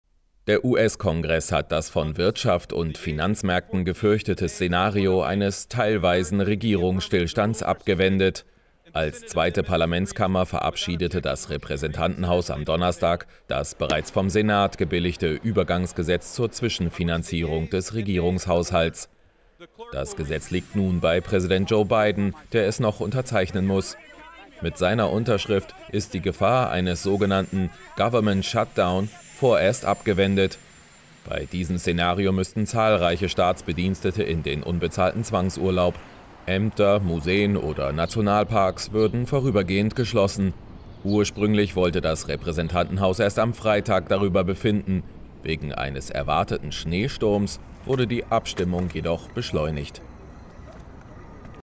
US-Kongress verhindert Regierungsstillstand
Das US-Repräsentantenhaus hat einen teilweisen Regierungsstillstand abgewendet. Das verabschiedete Übergangsgesetz zur Haushaltsfinanzierung liegt dem Präsidenten vor. Bei dem befürchteten "Government-Shutdown" hätten zahlreiche Staatsbedienstete in den unbezahlten Zwangsurlaub geschickt und Museen sowie Nationalparks vorübergehend geschlossen werden müssen.

Bildrechte Thumbnail: Leah Millis/Reuters